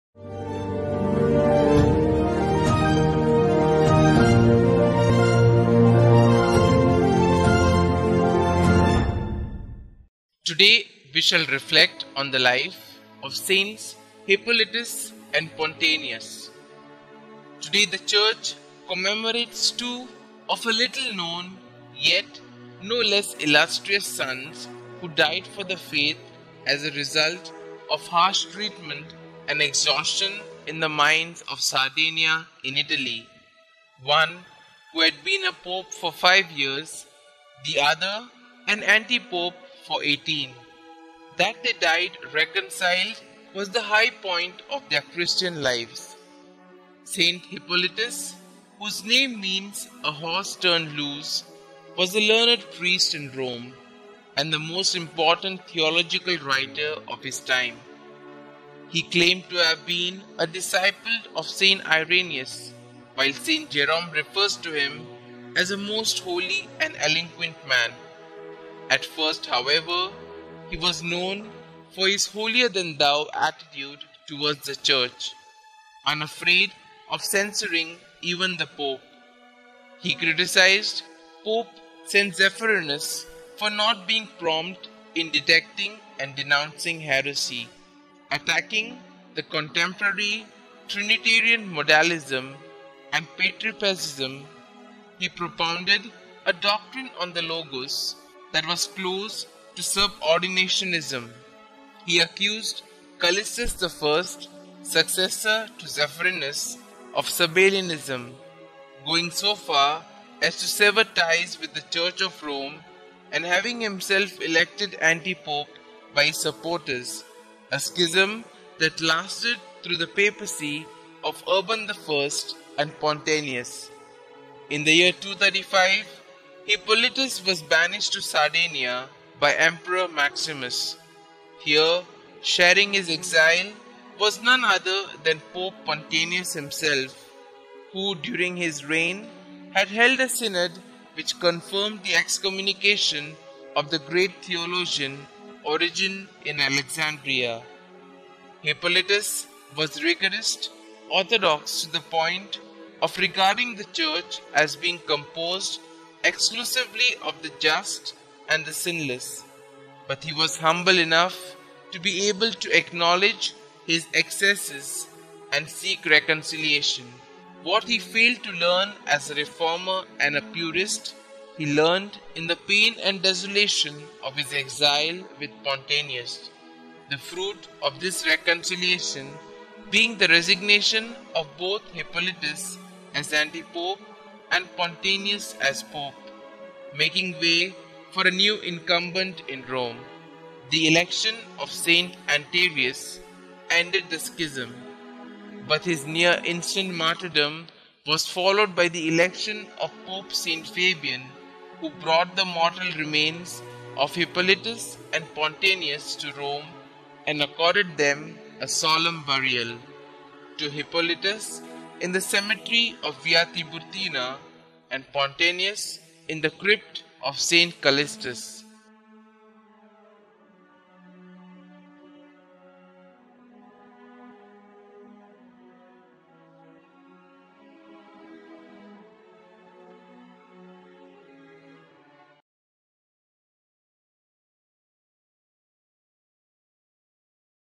Sts Hippolytus And Pontianus - Saint of the Day with Fr Lindsay - 13 August 2022
Here is a brief reflection on the life of Sts Hippolytus And Pontianus  presented by Fr Lindsay Lobo.